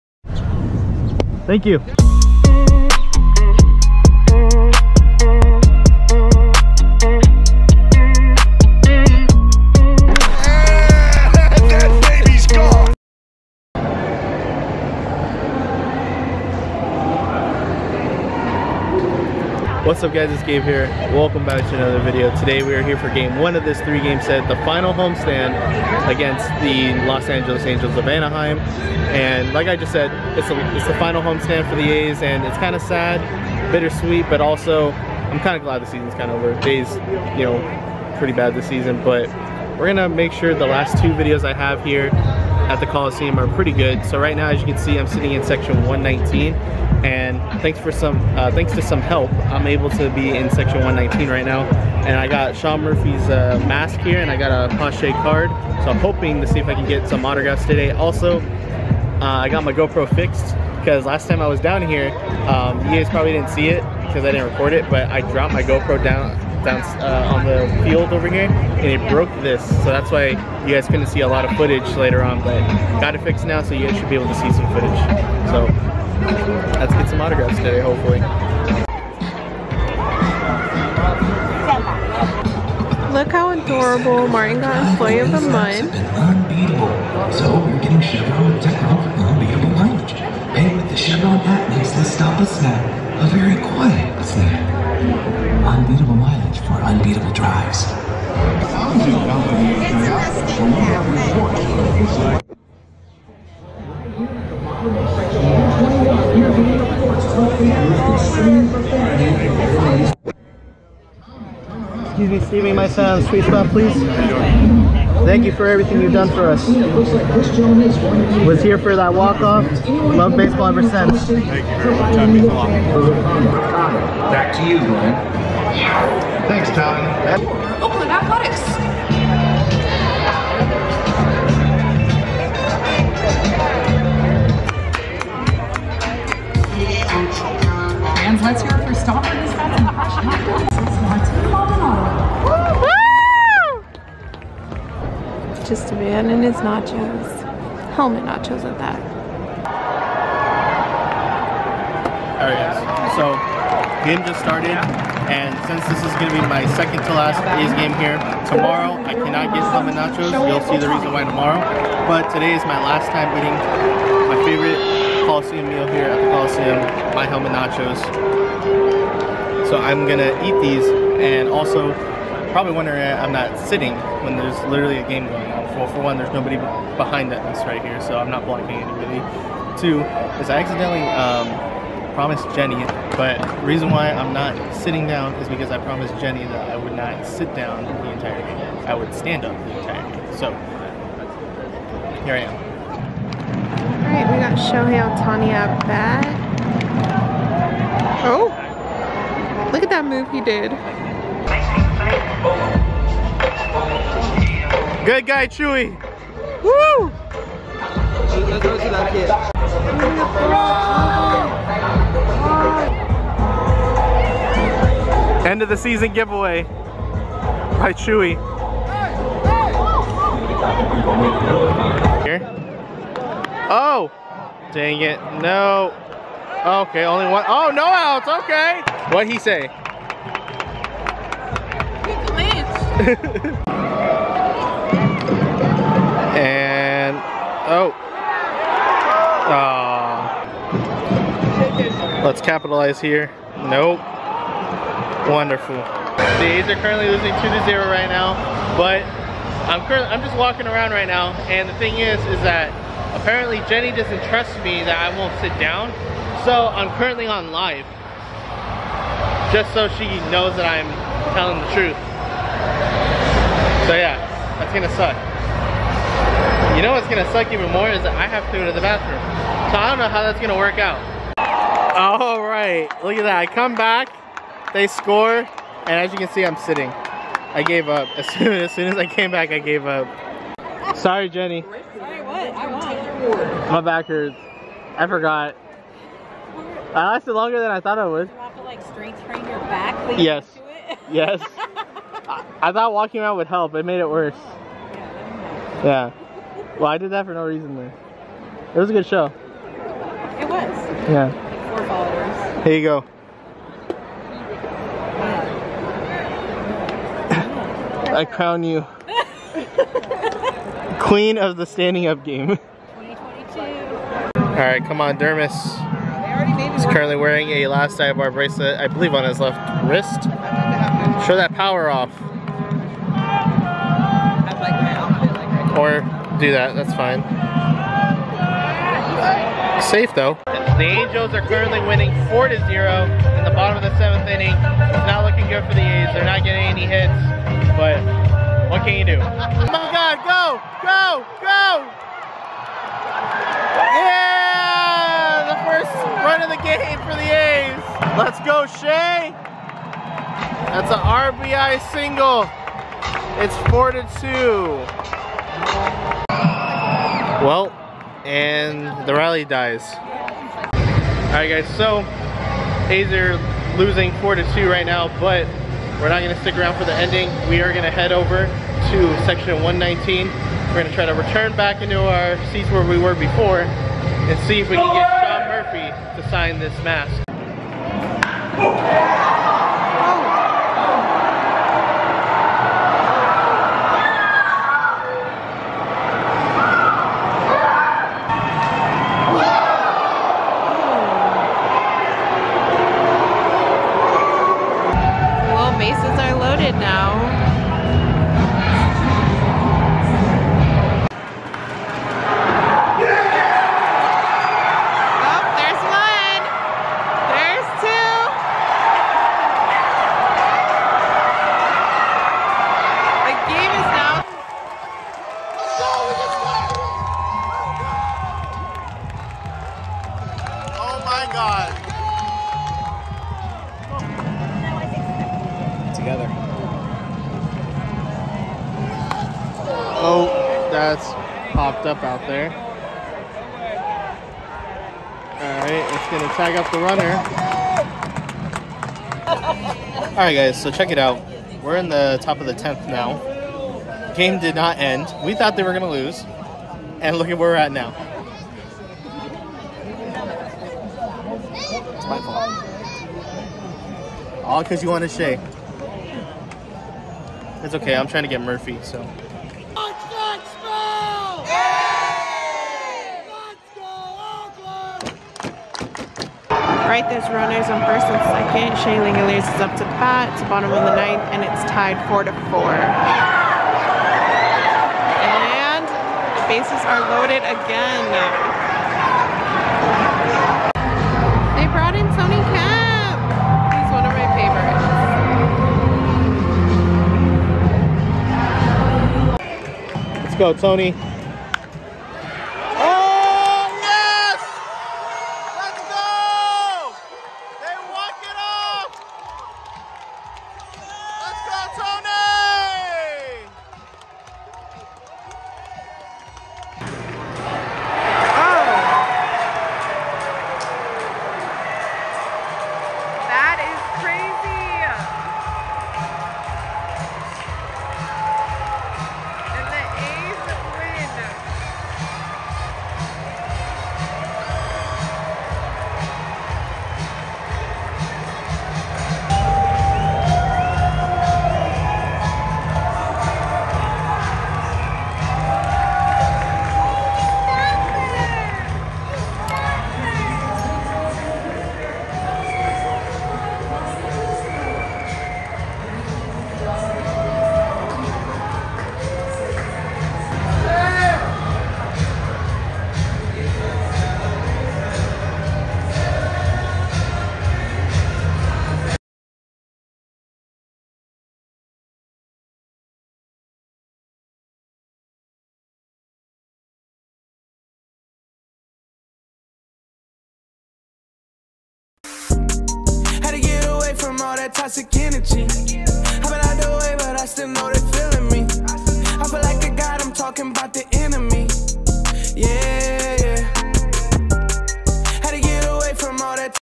Tony Calls Game
In this video, the A’s start their final homestand of the 2022 season

LINK TO LAST DIVE BAR MERCH!